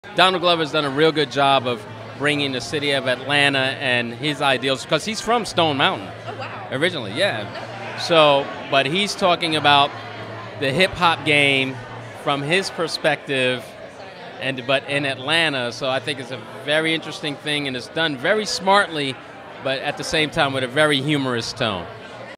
Interview with Lucius Baston
Join us as we interviewed some of Atlanta's finest actors at the opening night of La Tierra restaurant. We interviewed actor, Lucius Baston, from the hit tv show Atlanta. Catch him in his upcoming role on the show The Quad on BET.